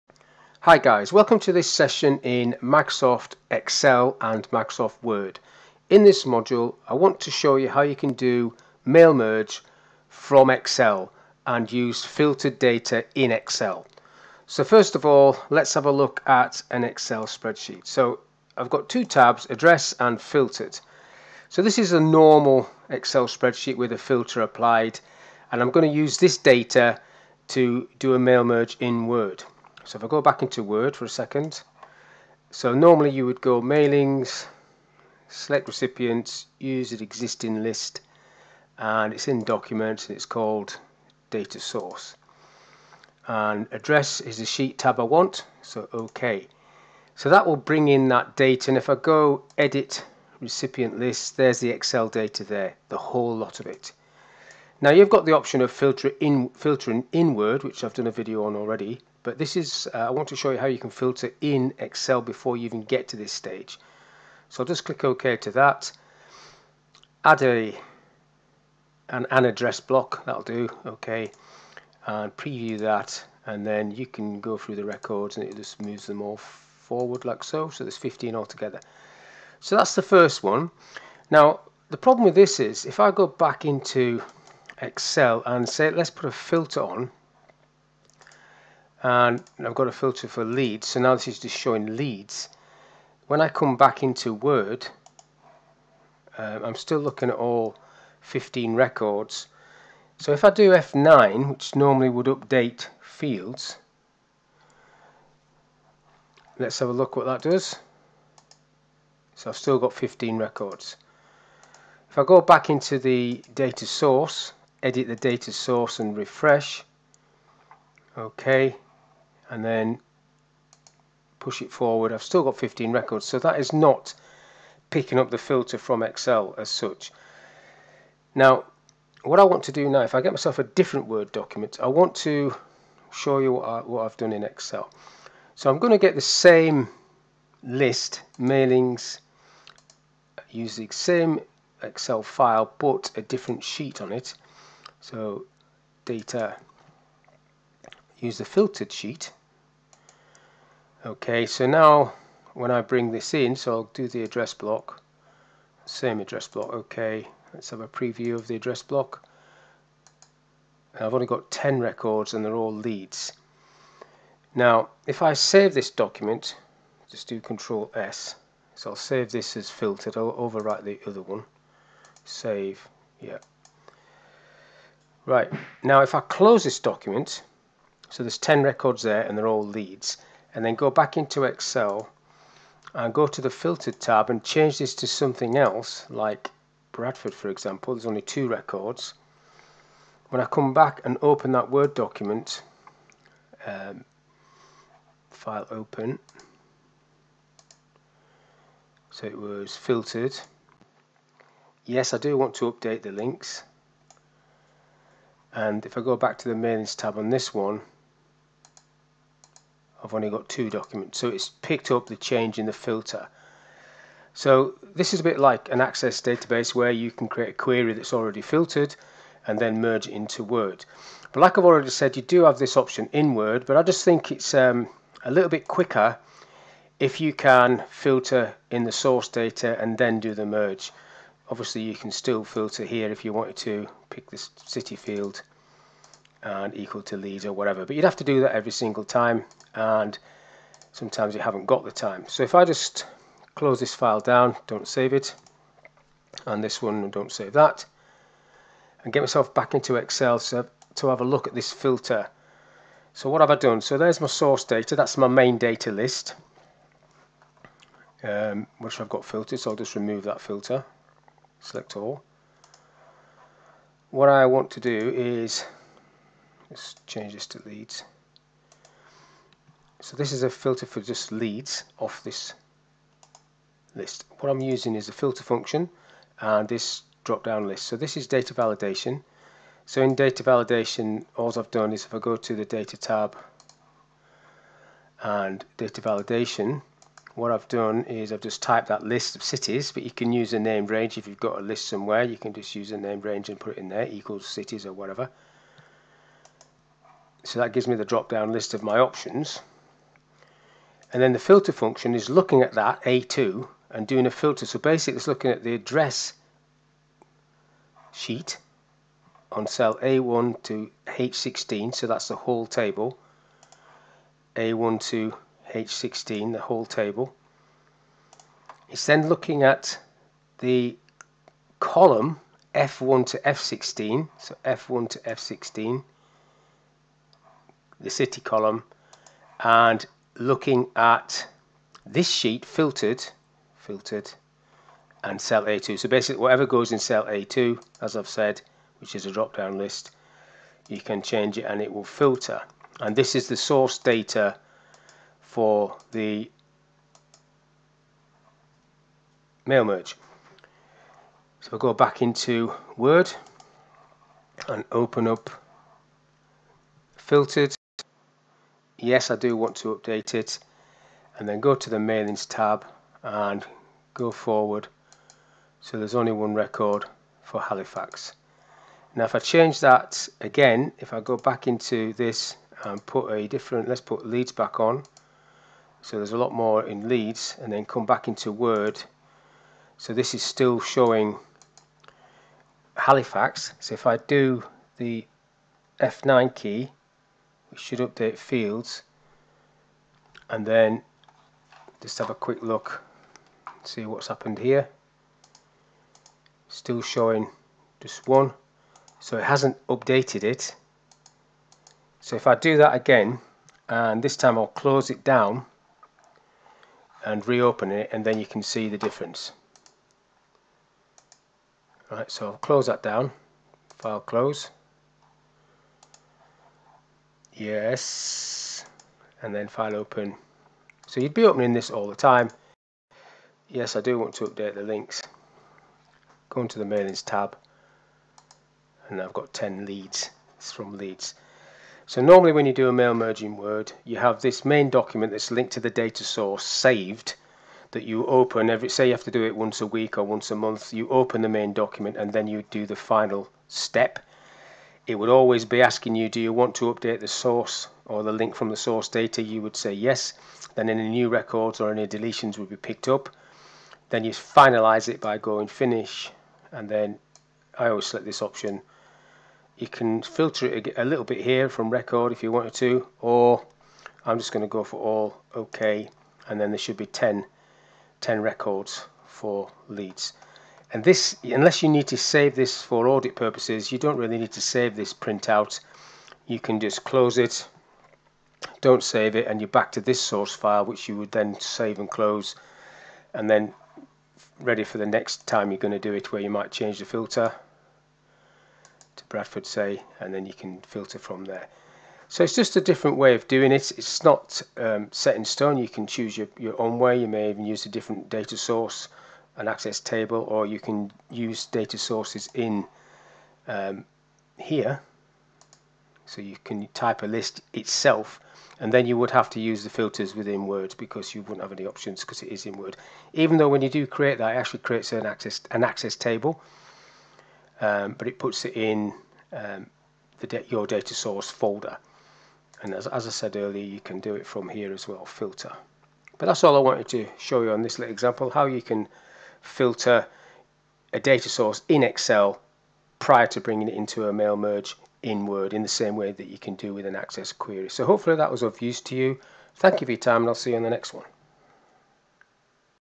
This video explains how to Mail merge from Excel. Filter and merge from Excel
This video explains how to filter records in Microsoft Excel before merging them into Microsoft Word’s mail merge feature. The tutor demonstrates how to create a data validation list in Excel and apply the filter function to react to any selection made from the list. Once the filter is applied the tutor explains how to use the filtered data as the data source for a merged document. The Word document refreshes data when opened displaying the new filtered data. This is a Microsoft Excel and Word tutorial covering, how to Mail merge from Excel, mail merge, Filter and merge from Excel, the filter function, data validation and data source.